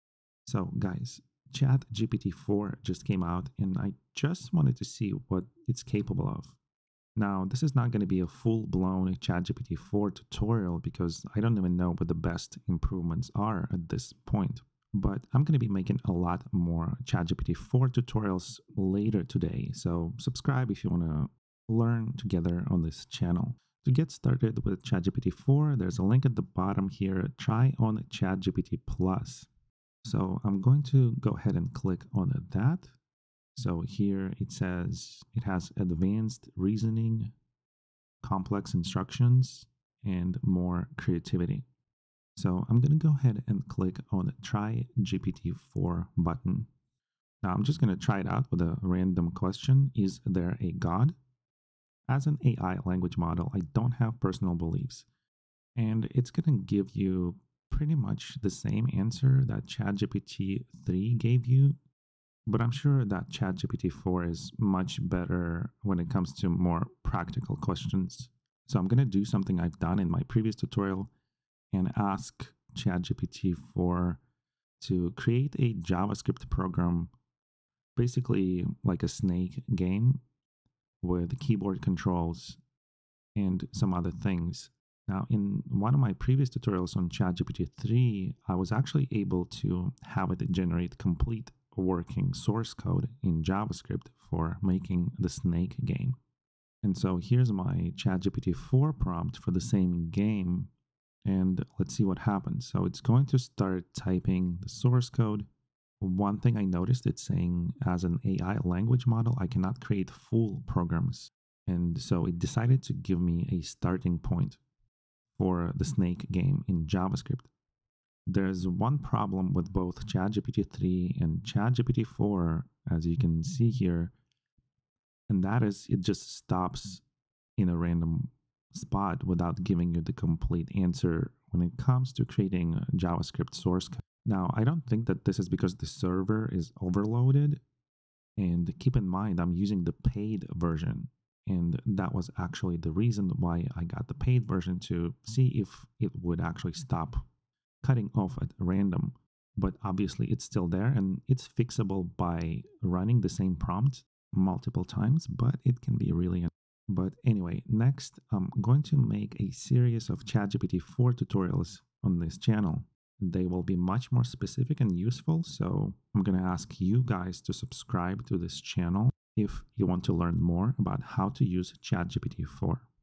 ChatGPT4 is here! What's new in Chat GPT-4?
So you heard the news, ChatGPT4 has come out today. And I'm starting a new tutorial series on seeing what's new and improved. This is just an intro tutorial to ChatGPT4 prompt. More is on the way.

00:00 ChatGPT4 Just Came Out (What's New?)
00:47 Writing first ChatGPT4 prompt
01:10 Ask ChatGPT4 to write source code for a simple JavaScript game